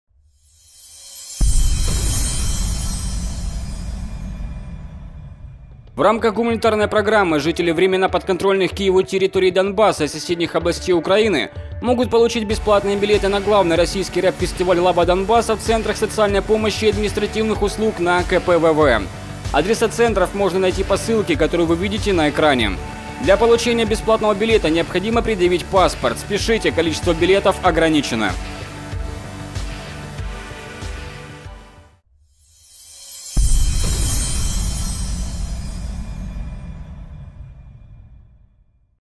Где получить билеты на "Лава Фест" для жителей временно подконтрольных Киеву территорий
В рамках Гуманитарной программы жители временно подконтрольных Киеву территорий Донбасса и соседних областей Украины могут получить бесплатные билеты на главный российский рэп-фестиваль "Лава Фест" в центрах социальной помощи и административных услуг на КПВВ.
Адреса центров можно найти по ссылке, которую вы видите на экране.
Для получения бесплатного билета необходимо предъявить паспорт.
Спешите, количество билетов ограничено.